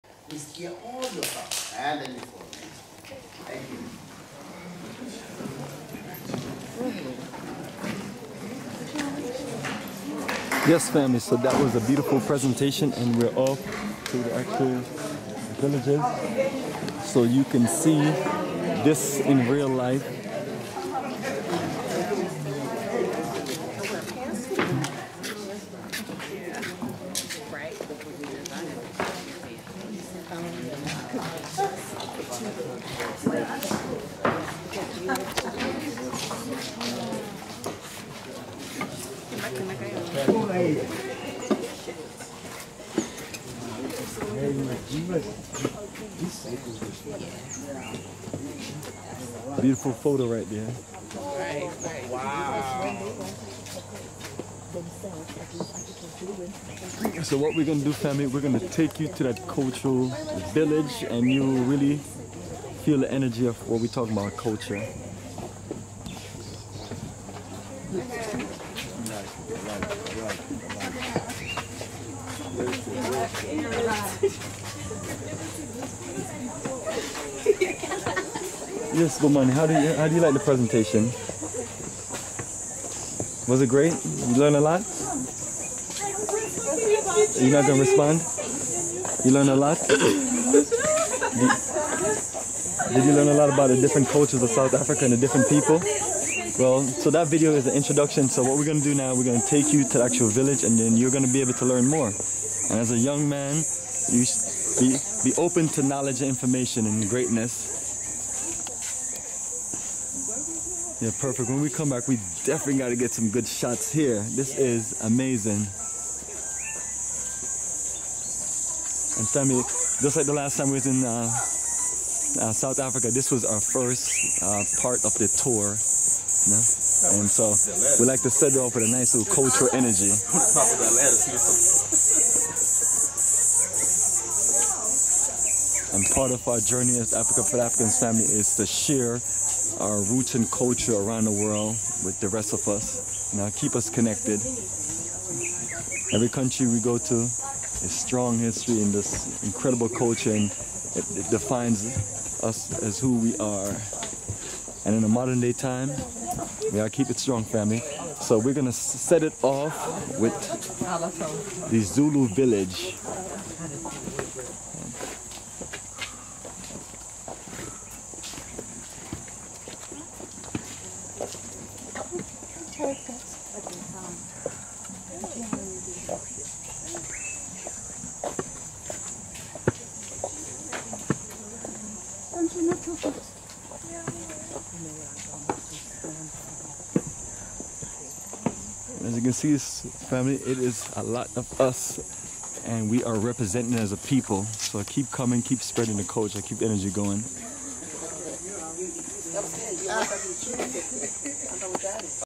Off to Start Village Tours at Lesedi Cultural Village - South Africa Dec 2023 Journey of a Lifetime
Highlights from our South Africa Dec 24, 2023 - Jan 4, 2024, Roots & Culture Journey of a Lifetime Tour. This adventure takes you throughout Johannesburg, Soweto, Pilanesberg & Cape Town. We are celebrating 17 Years of Africa for the Africans Tours & Investments from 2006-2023. This is on our Second South Africa Journey of a Lifetime.